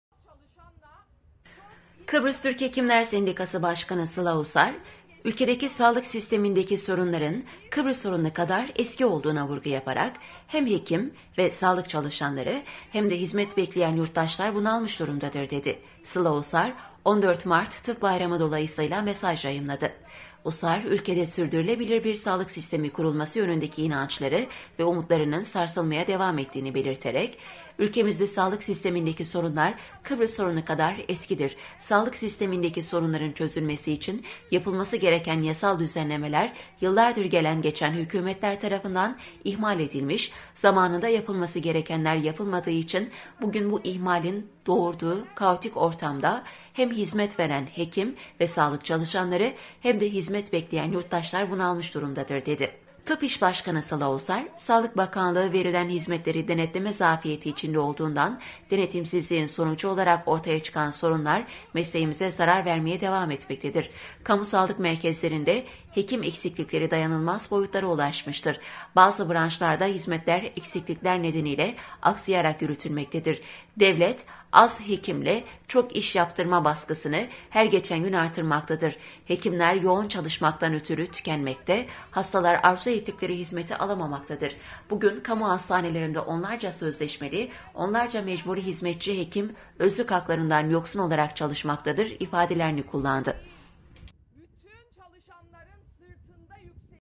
SILA USAR: “SAĞLIK SİSTEMİNDEKİ SORUNLAR KIBRIS SORUNU KADAR ESKİDİR”
Kıbrıs Türk Hekimler Sendikası Başkanı Sıla Usar, ülkedeki sağlık sistemindeki sorunların Kıbrıs sorunu kadar eski olduğuna vurgu yaparak, “Hem hekim ve sağlık çalışanları, hem de hizmet bekleyen yurttaşlar bunalmış durumdadır” dedi.   Sıla Usar, 14 Mart Tıp Bayramı dolayısı ile mesaj yayınladı.  Usar, ülkede sürdürülebilir bir sağlık sistemi kurulması yönündeki inançları ve umutlarının sarsılmaya devam ettiğini belirterek, “Ülkemizde sağlık sistemindeki sorunlar Kıbrıs sorunu kadar eskidir. Sağlık sistemindeki sorunların çözülmesi için yapılması gereken yasal düzenlemeler yıllardır gelen geçen hükümetler tarafından ihmal edilmiş, zamanında yapılması gerekenler yapılmadığı için bugün bu ihmalin doğurduğu kaotik ortamda hem hizmet veren hekim ve sağlık çalışanları, hem de hizmet bekleyen yurttaşlar bunalmış durumdadır” dedi.  Tıp-İş Başkanı Sıla Usar, “Sağlık Bakanlığı verilen hizmetleri denetleme zafiyeti içinde olduğundan denetimsizliğin sonucu olarak ortaya çıkan sorunlar mesleğimize zarar vermeye devam etmektedir. Kamu sağlık merkezlerinde hekim eksiklikleri dayanılmaz boyutlara ulaşmıştır. Bazı branşlarda hizmetler eksiklikler nedeni ile aksayarak yürütülmektedir. Devlet az hekimle çok iş yaptırma baskısını her geçen gün artırmaktadır. Hekimler yoğun çalışmaktan ötürü tükenmekte, hastalar arzu ettikleri hizmeti alamamaktadır. Bugün kamu hastanelerinde onlarca sözleşmeli, onlarca mecburi hizmetçi hekim özlük haklarından yoksun olarak çalışmaktadır” ifadelerini kullandı.